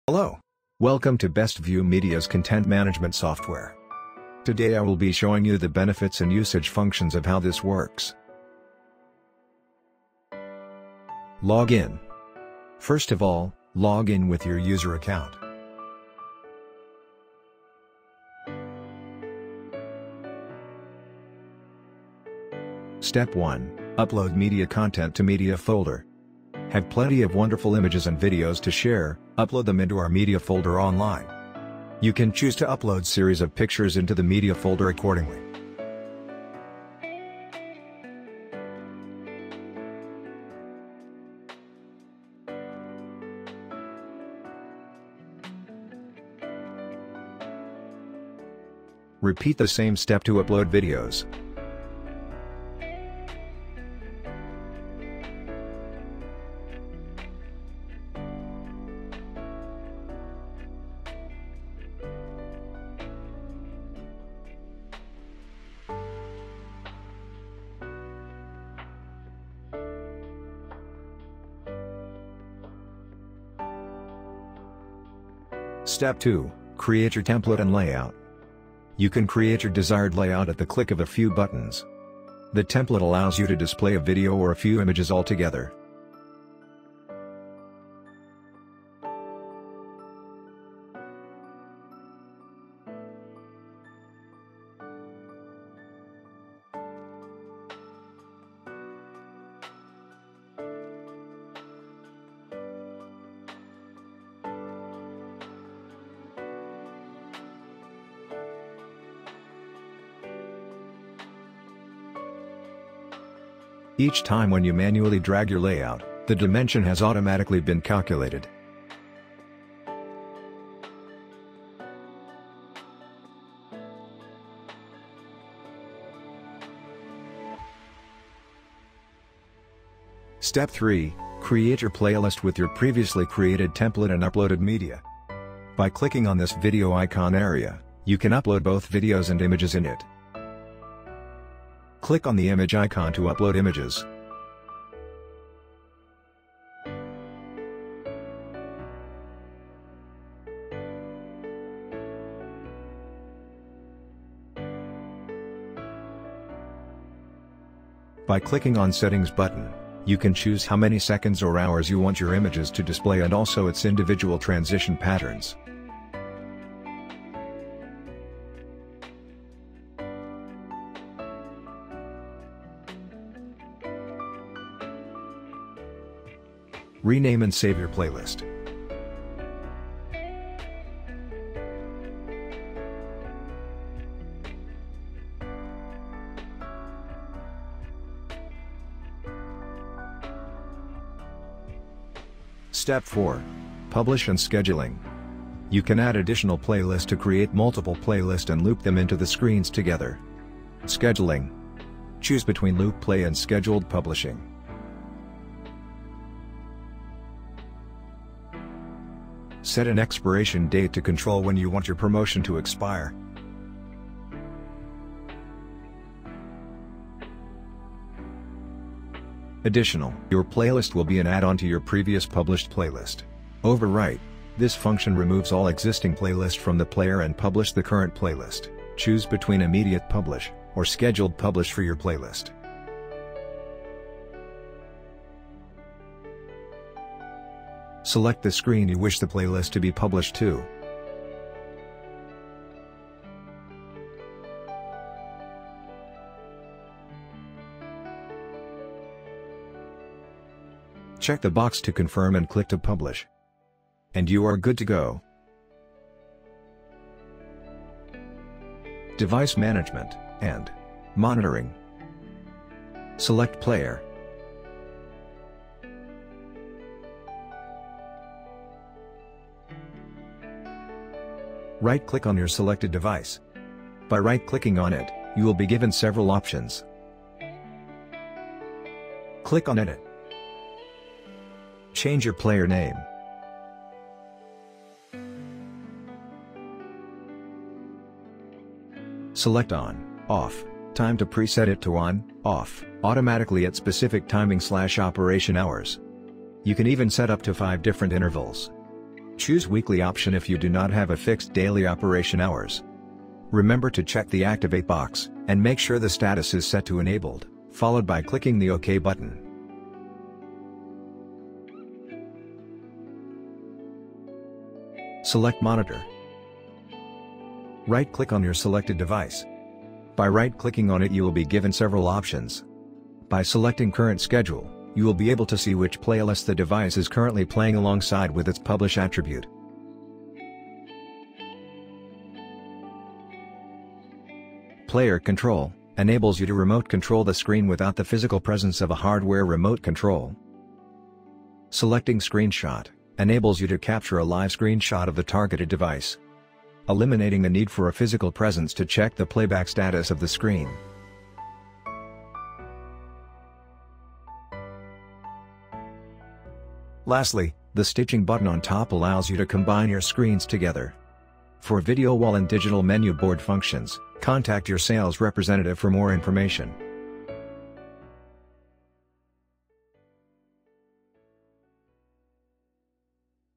BEST VIEW MEDIA CDMS KNOW HOW
Digital Signage Know How.
This video gives you a head start on how to use your CDMS account
Introduction to CDMS
Logging In, Creating and designing your layout
Storing your content on cloud
Creating a special play list and
Scheduling your content.